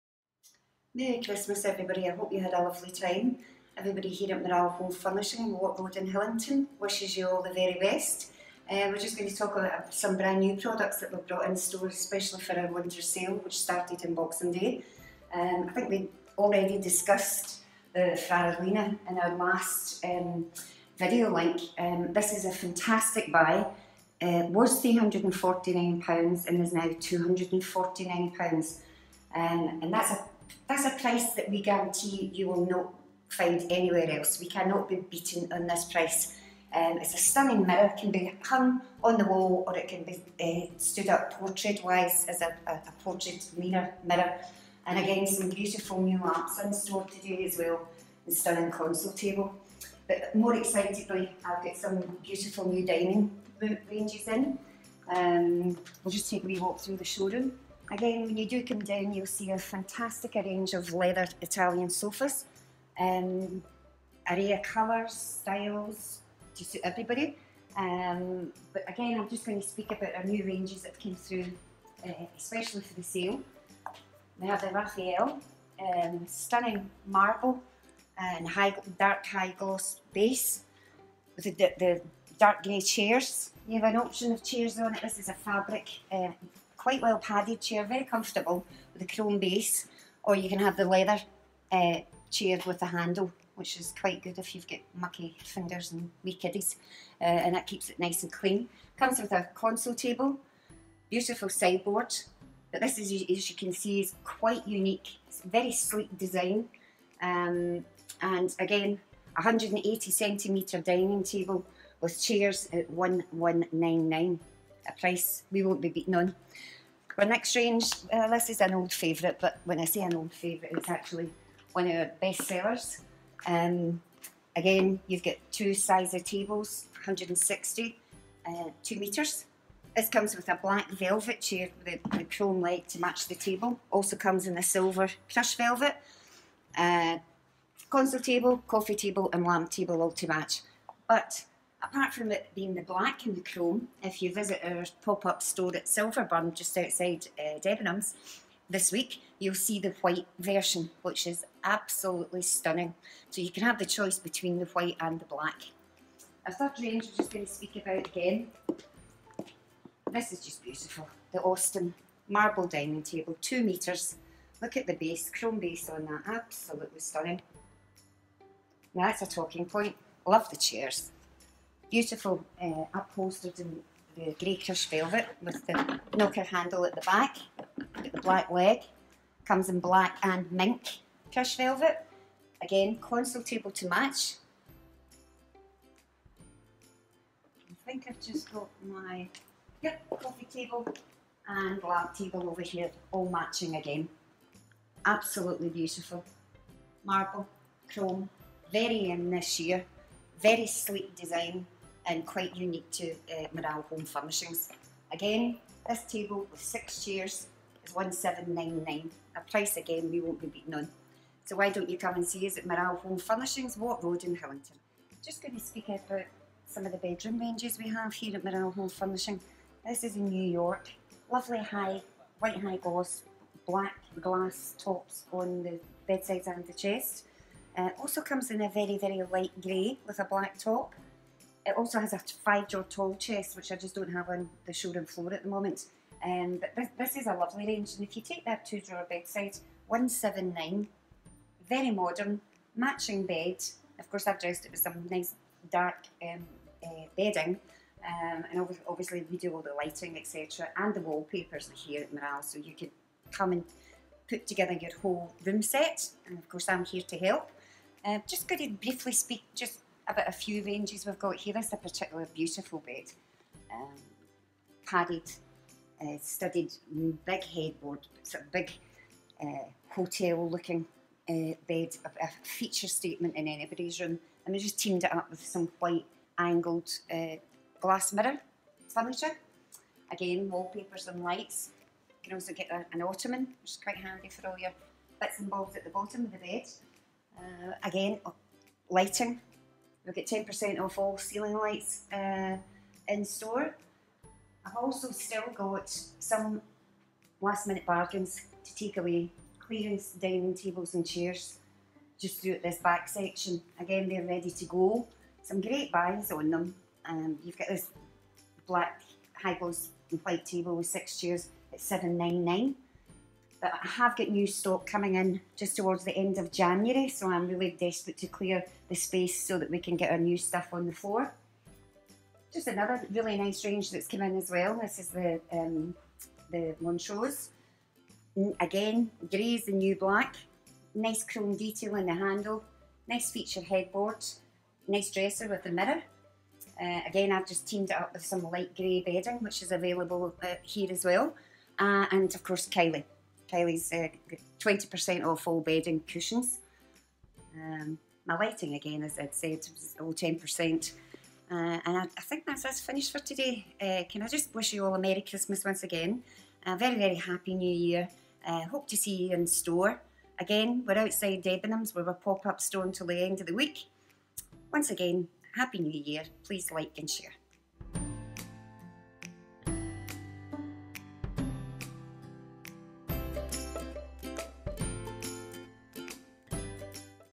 Winter Sale Now On at Morale Home Furnishings!
Big Winter Sale Now On at Morale Home Furnishings! All items on sale with massive savings of up to 75% plus more! Plus all clearance items must go!! Come see Christine today and grab a bargain!